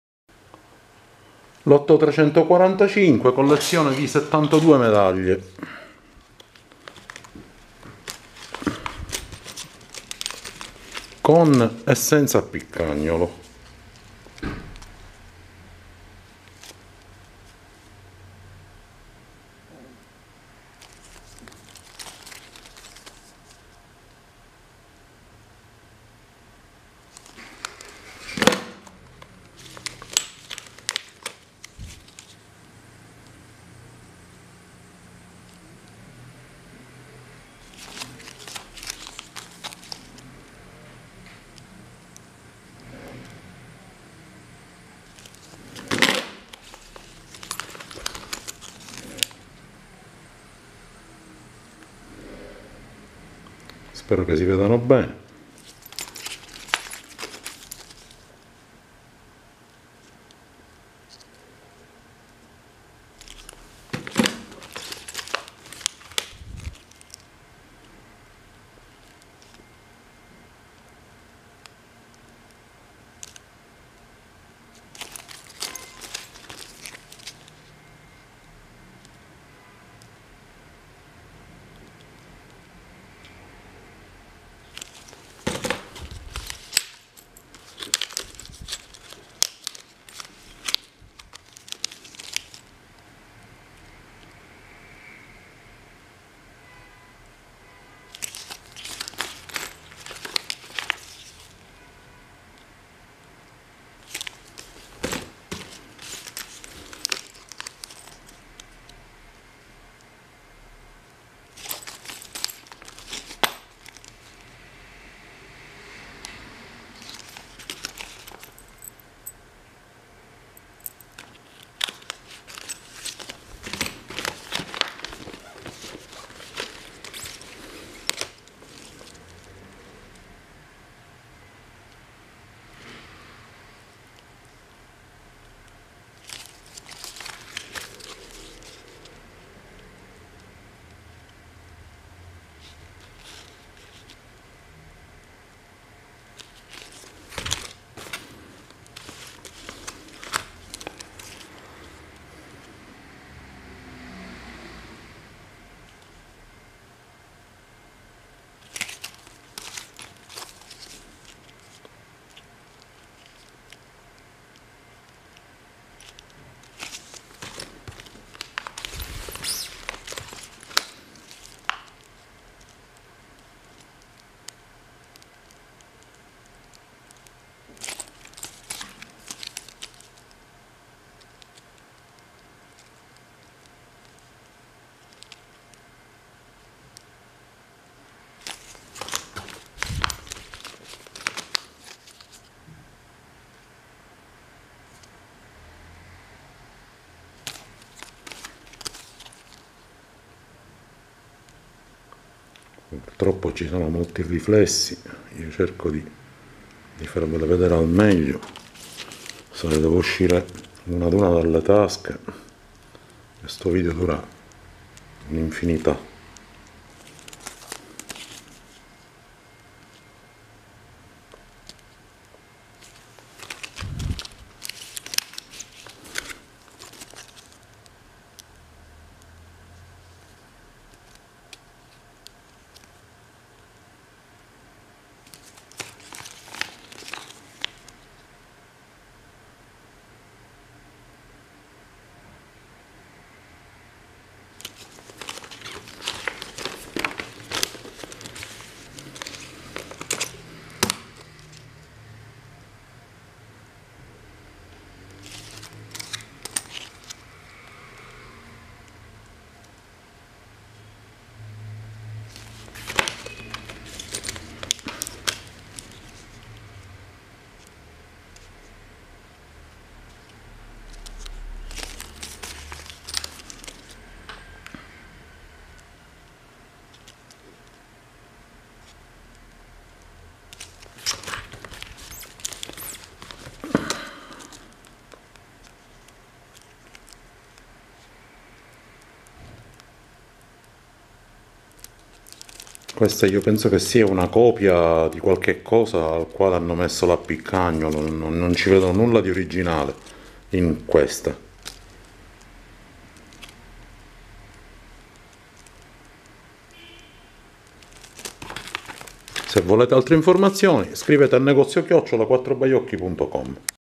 Lotto 345 - Medaglie - Collezione di 72 esemplari XIX e XX secolo
Medaglie - Collezione di 72 esemplari XIX e XX secolo in un album.